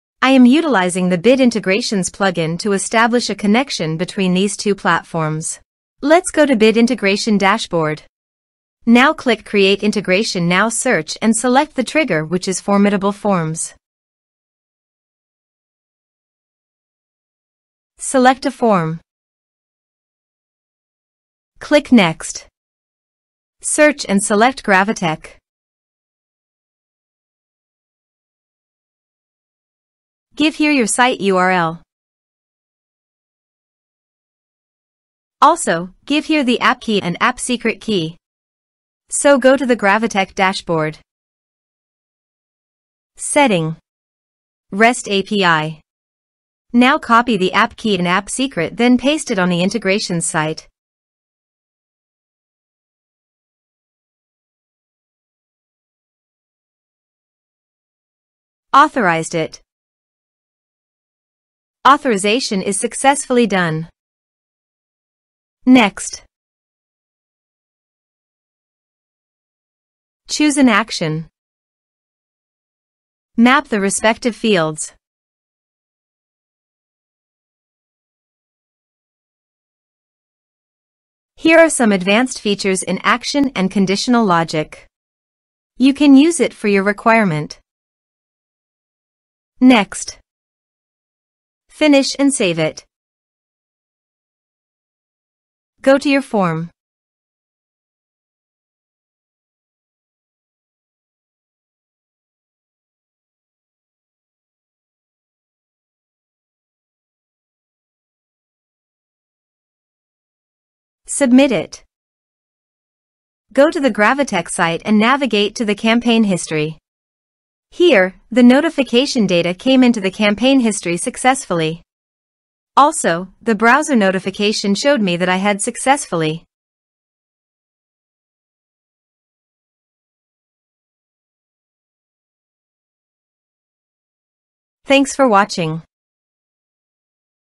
Integrating Formidable Forms with Gravitec | Step-by-Step Tutorial | Bit Integrations
🔗 Ready to optimize your email marketing strategy? Join us on our YouTube channel for an in-depth tutorial where we reveal the seamless integration of Formidable Forms with Gravitec, a powerful email marketing platform. Learn how to effortlessly connect these two tools using the Bit Integrations WordPress plugin.

Follow along as we guide you through the step-by-step process of integrating Formidable Forms with Gravitec, empowering you to enhance your email marketing campaigns and increase subscriber engagement. Bid farewell to manual data transfers and welcome automated workflows for improved email marketing efficiency.

Key Highlights:
✅ In-depth walkthrough for integrating Formidable Forms with Gravitec using Bit Integrations for WordPress.
✅ Leverage your form data to create engaging and targeted email campaigns with Gravitec.
✅ Elevate your email marketing strategy with streamlined automation and increased productivity.

Ready to revolutionize your email marketing game?

Unlock advanced email marketing capabilities:
📧 Elevate your email marketing endeavors with the Pro version of Bit Integrations

Whether you're a marketer, business owner, or email enthusiast, this integration empowers you to seamlessly merge Formidable Forms with Gravitec. Watch now to revolutionize your email marketing efforts!

🎥 Ready to revolutionize your email marketing? Join us in optimizing your email campaigns with seamless integration! 👍